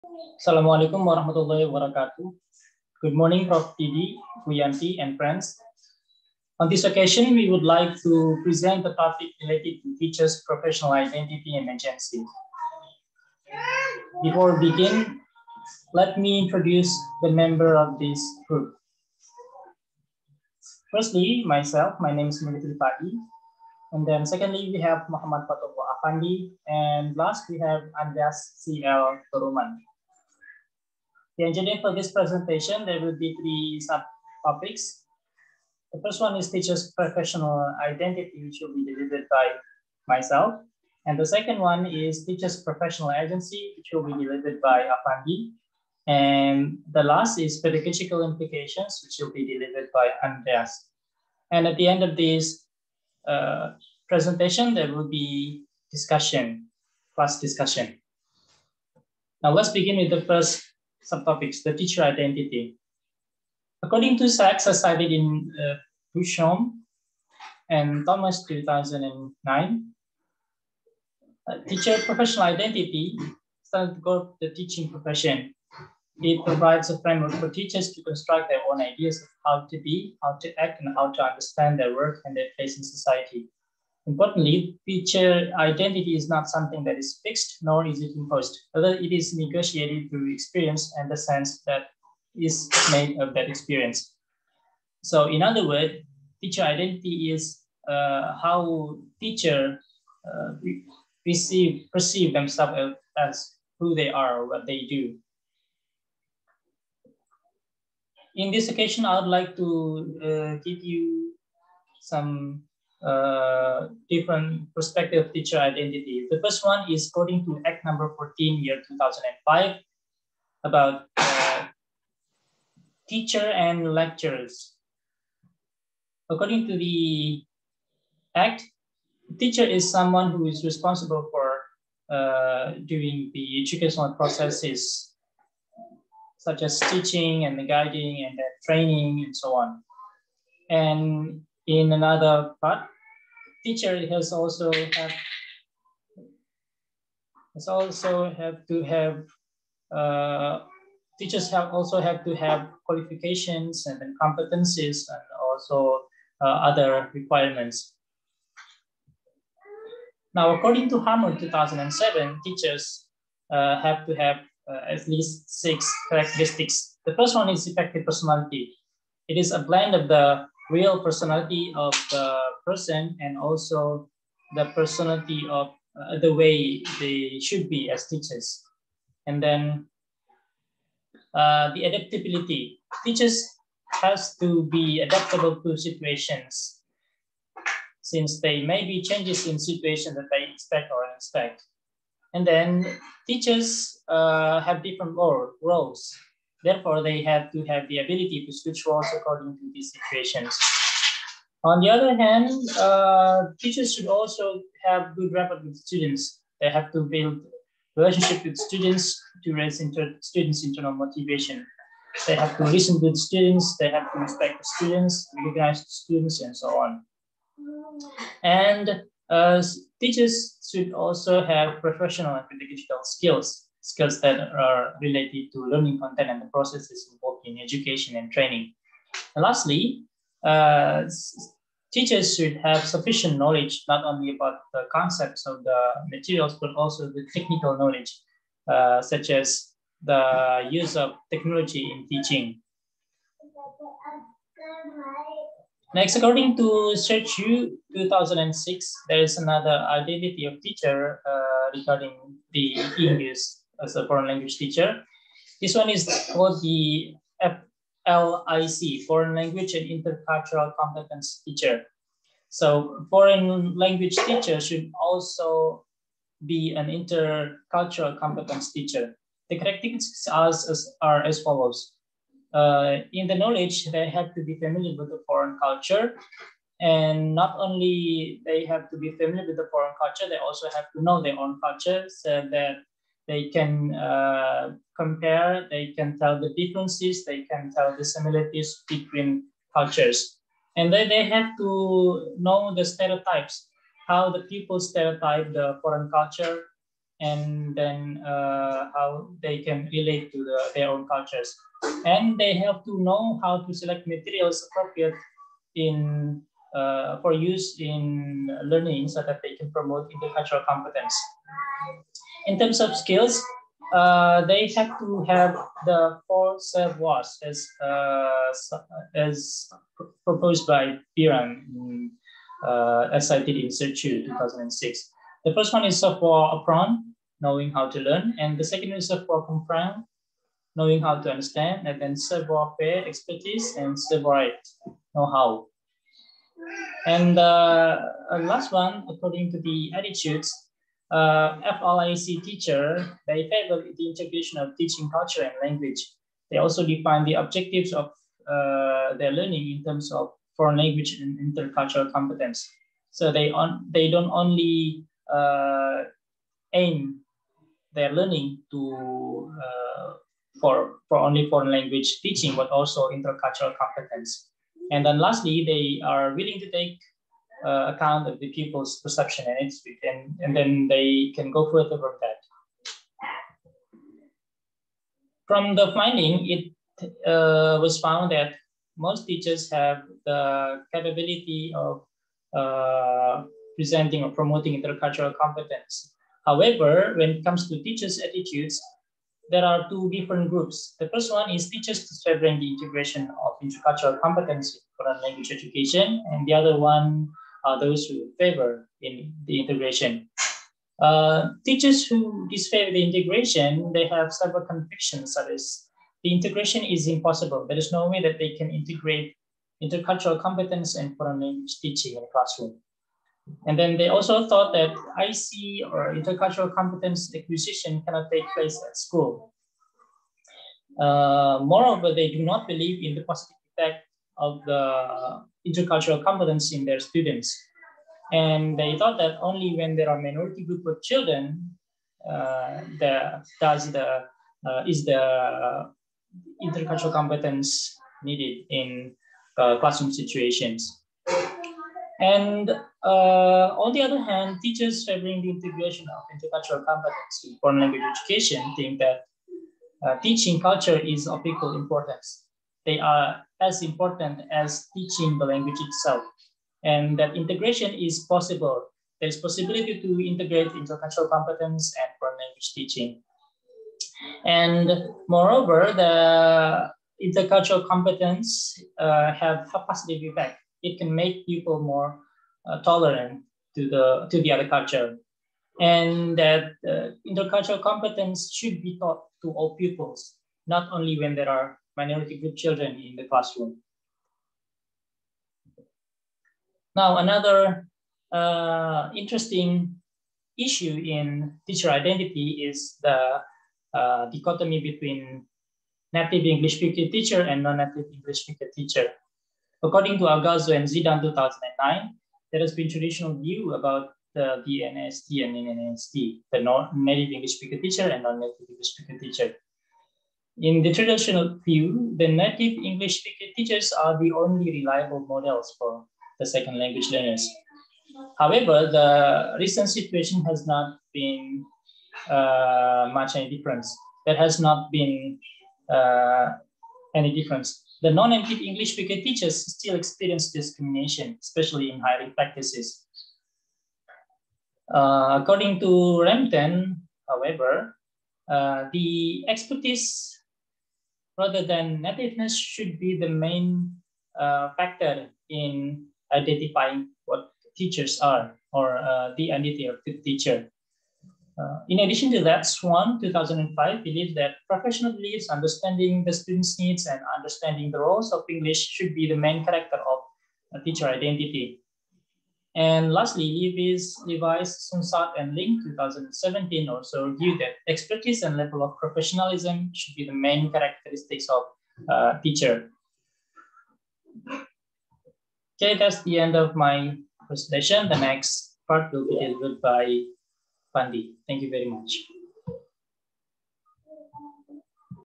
EFL English Teachers' Identity
My presentation on the topic of EFL Teachers' Identity:
- Definition of identity
- Teachers' identity according to Indonesian Act no. 14 year 2005
- Teachers' identity according to Jeremy Harmer, 2007
- Foreign language and intercultural competence teachers' identity according to Sercu, 2006
- The dichotomy between Native English Speaker Teachers and Non-native Englsih Speaker Teachers according to Alghazo and